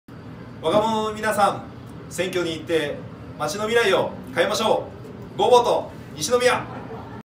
Govote NISHINOMIYA投票行こう！西宮！キャンペーンvol.104
18 歳から24 歳の西宮在住の皆様へ。
選挙へ行く！Web 署名はこちら。GoVoteNISHINOMIYA
西宮青年会議所が主催する「Go Vote NISHINOMIYA ～投票行こう！西宮！キャンペーン」です。

今回は建築業、橋本さんです！

兵庫県議会議員選挙、兵庫県についての詳細や、